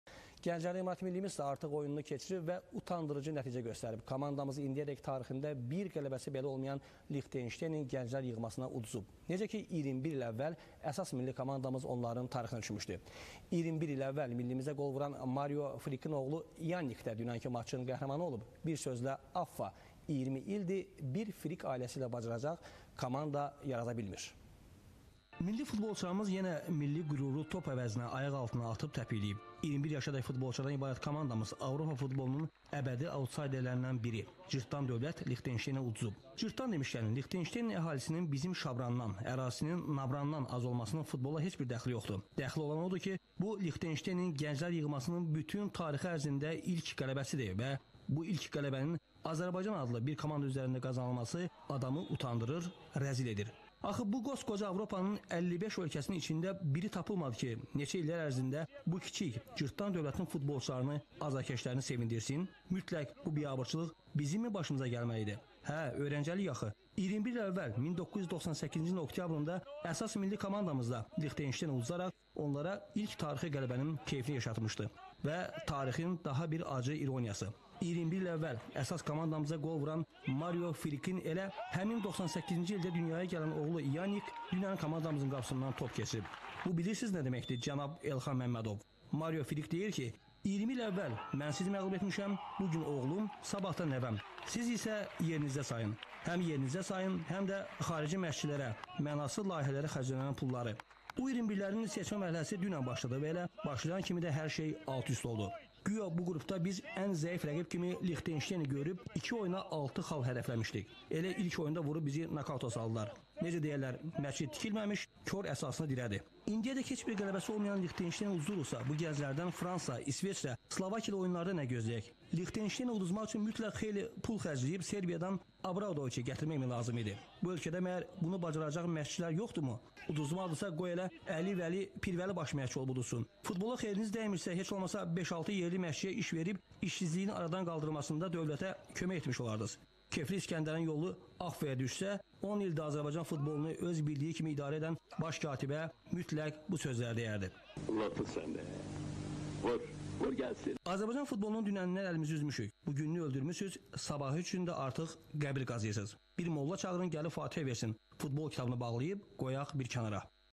ILKIN XELILOV AZERBAYCAN MILLISINI YENE SERT TENQID ETDI 07.06.2019
Lixtenstein 1-0 Azerbaycan U21Avropa Cempionati Secme Merhelesi